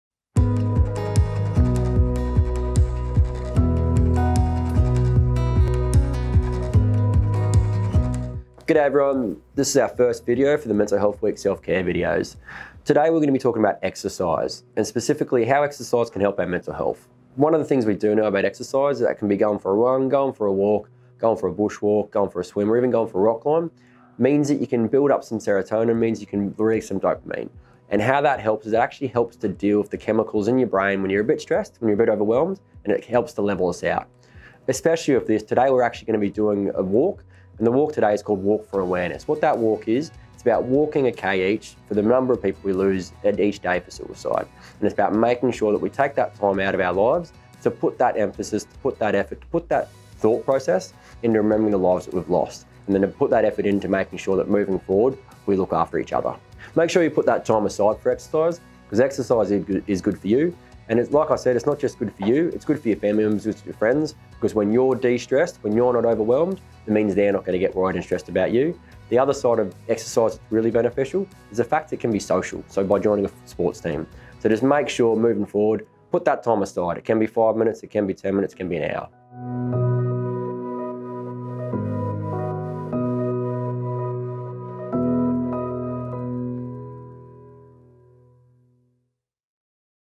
MATES in Construction Mental Health Week 2022 - Daily tips for BetterMentalHealth
For Queensland Mental Health Week 2022, we will be sharing daily tips and ideas that can have a positive impact on our wellbeing.

Today's video also coincides with Walk for Awareness while we are talking about exercise while we also celebrate WalkforAwareness.
Be sure to stop in each day for our short video messages with ideas on how to stay happy and healthy.

Remember, if you see a MATE doing it tough, reach out to see how they are travelling and connect them to help.

MATES Stronger Together
MATES 24/7: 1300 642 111